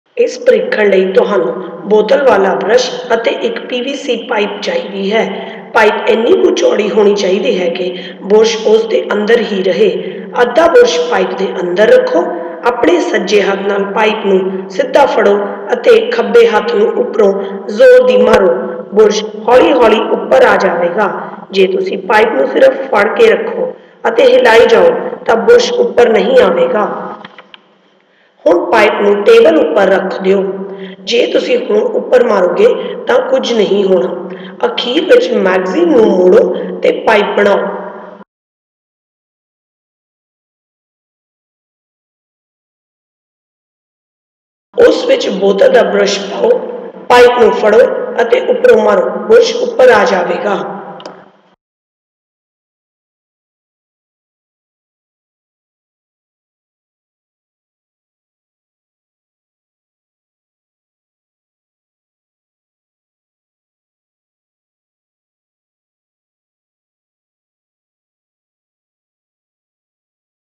Climbing brush | Punjabi
For this experiment you will need a Bottle Brush and a PVC pipe. The pipe diameter should be such that the brush must just stay in it.
Place half Brush in Pipe. Hold pipe vertically with right hand and hit hard on top with the left hand. The brush will slowly climb up in the pipe.
If you just hold the pipe and give it jerks nothing happens.
Now rest the pipe on a table. If you tap now the brush does not climb.

Finally roll a magazine to make a pipe. Place the Bottle Brush in it.
Hold the rolled pipe and tap from top. The brush will climb.